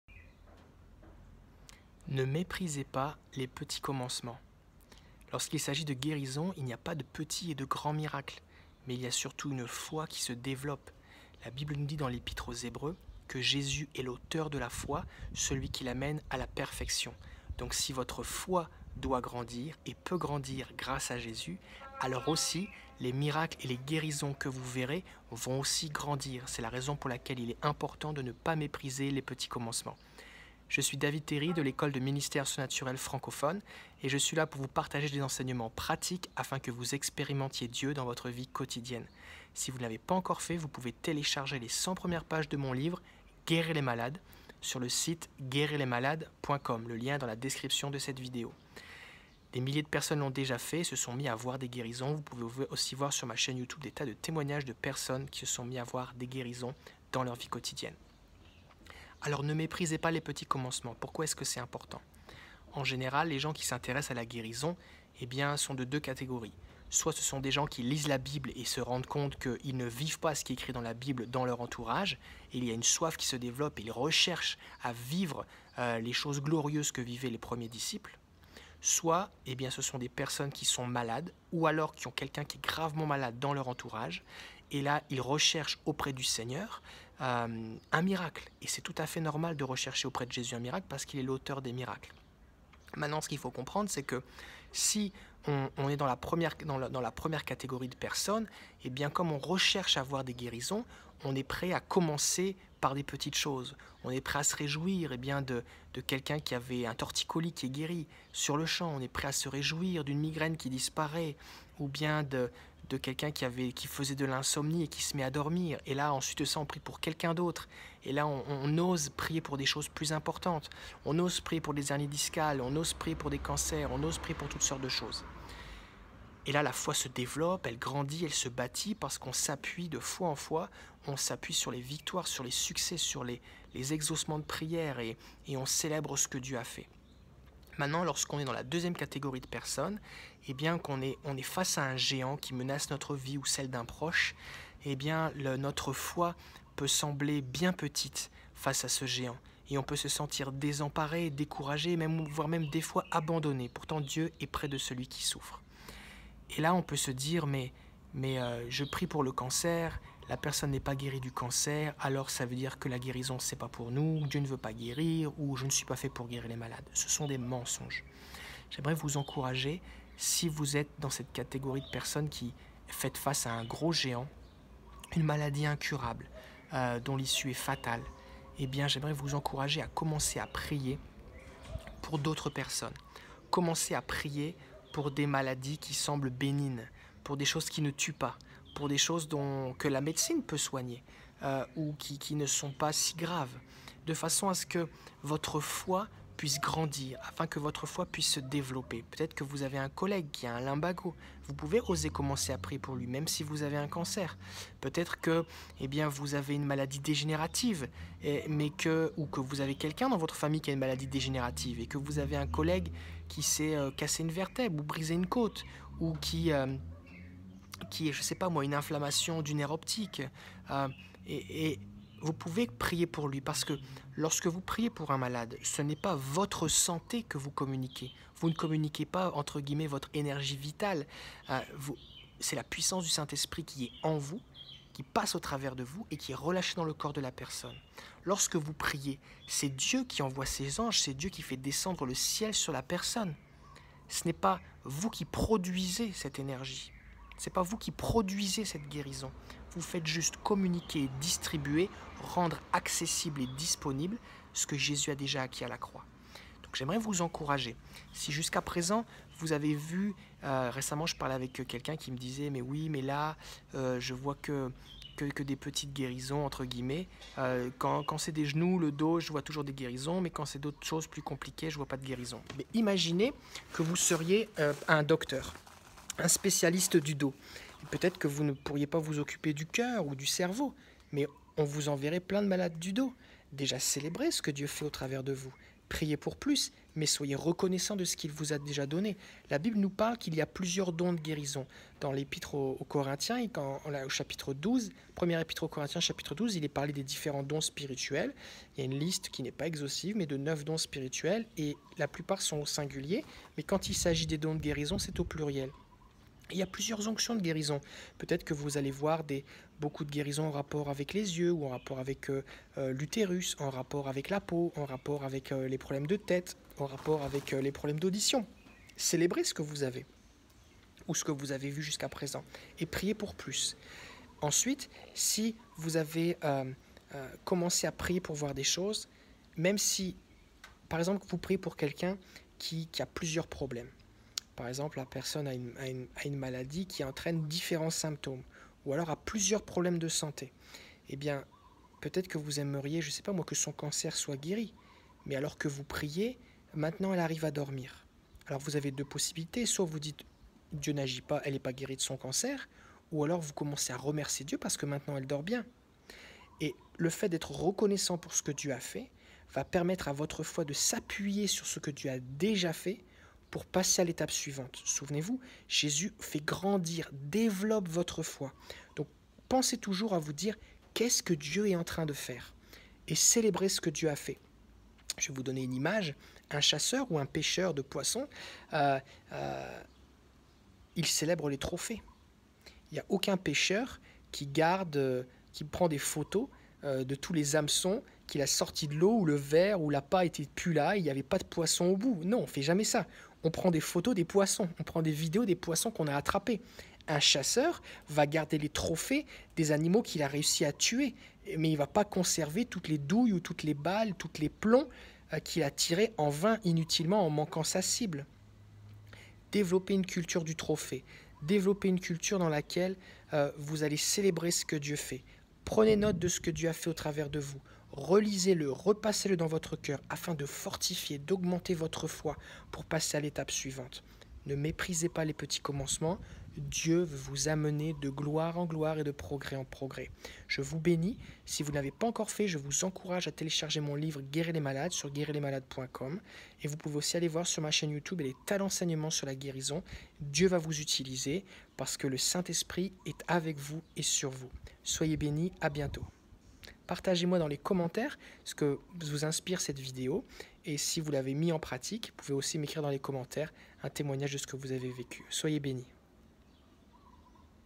Ne méprisez pas les petits commencements pour la guérison !
Développez votre foi pas à pas et découvrez comment célébrer chaque miracle, même petit, pour grandir dans le domaine de la guérison divine

0:00 Introduction : Ne Méprisez Pas les Petits Commencements
0:21 Pourquoi la Foi et les Miracles Grandissent
1:01 Les Deux Catégories de Personnes Concernées par la Guérison
2:00 Comment Développer sa Foi par des Petites Victoires
2:19 Faire Face à de Grands Défis avec une Petite Foi
2:51 Priez pour des Cas Simples pour Faire Grandir Votre Foi
3:52 La Puissance du Saint-Esprit dans la Guérison
4:22 Célébrez les Guérisons et les Dons que Dieu Vous Donne
5:35 Remerciez Dieu pour les Progrès Partiels
6:17 Adoptez une Culture du Trophée Spirituel

📖 Pour vous procurer le livre GUÉRIR LES MALADES au format livre ou eBook :

🙏Télécharger gratuitement le Guide de prière pour les malades en 5 étapes